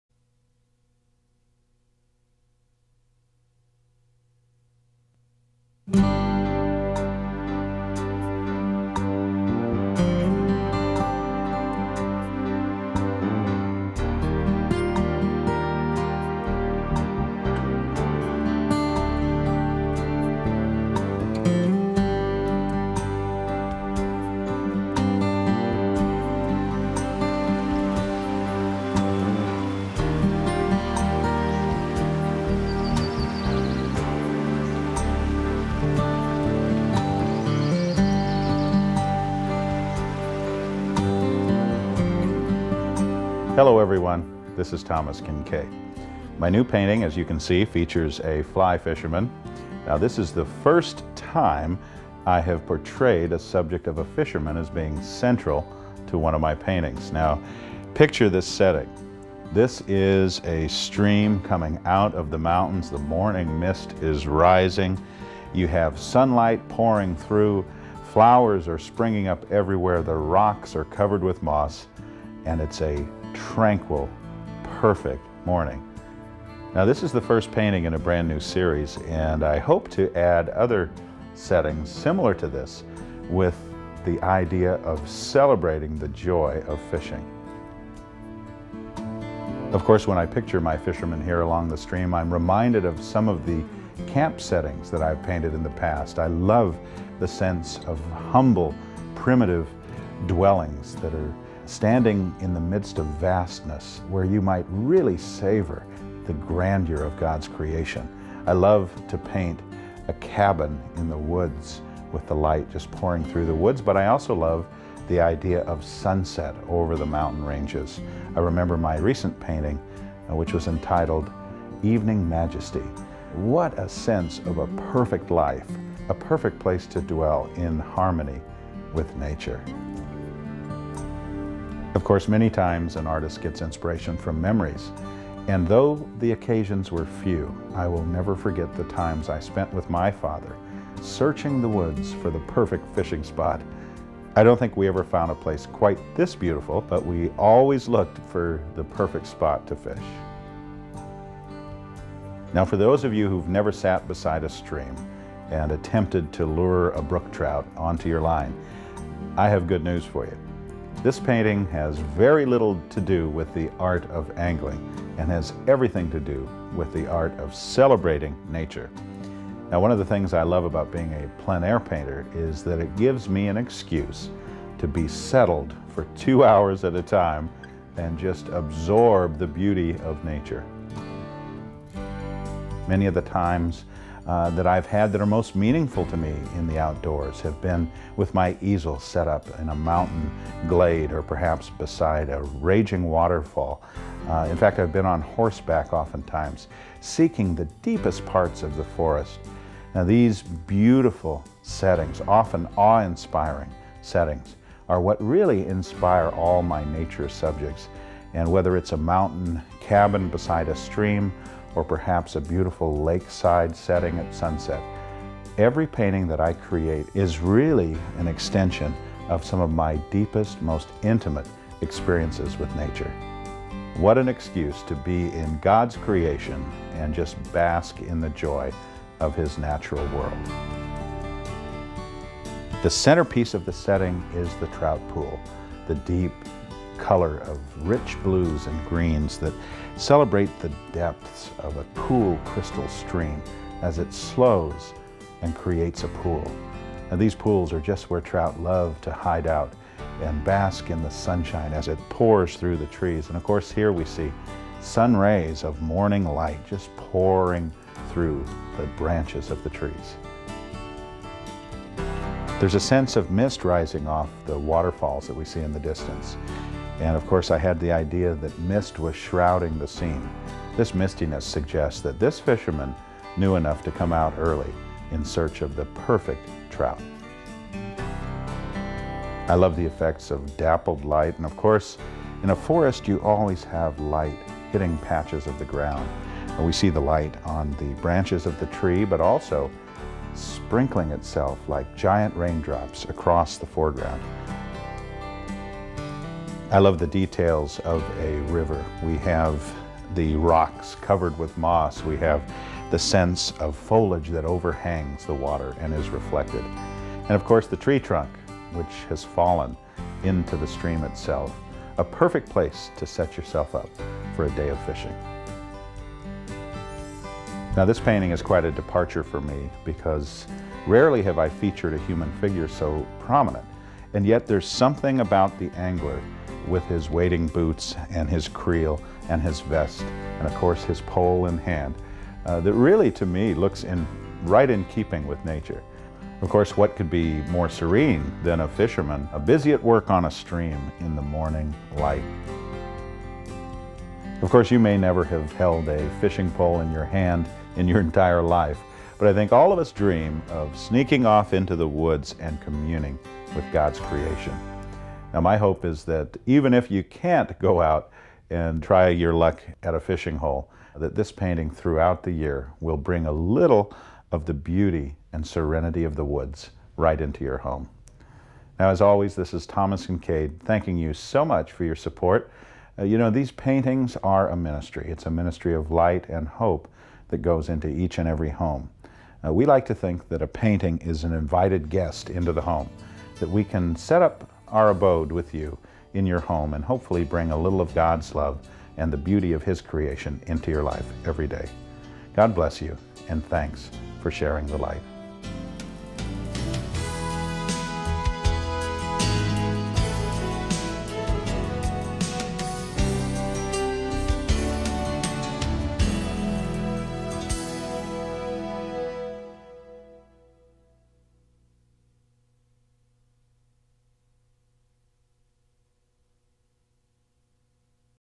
It Doesn't Get Much Better by Thomas Kinkade
Like the great American realist Winslow Homer, I have a passion for fishing. My best "catches" are often the superb vistas I discover on fishing trips. It Doesn't Get Much Better makes the point. When you stumble on a breathtaking fishing hole like this on a mist-drenched morning, it hardly matters whether they're biting or not. When, as in my very hopeful canvas, the stately fisherman, properly outfitted in full waders, hooks onto a feisty rainbow trout, it truly doesn't get much better than this. Even Winslow Homer would have agreed with that sentiment.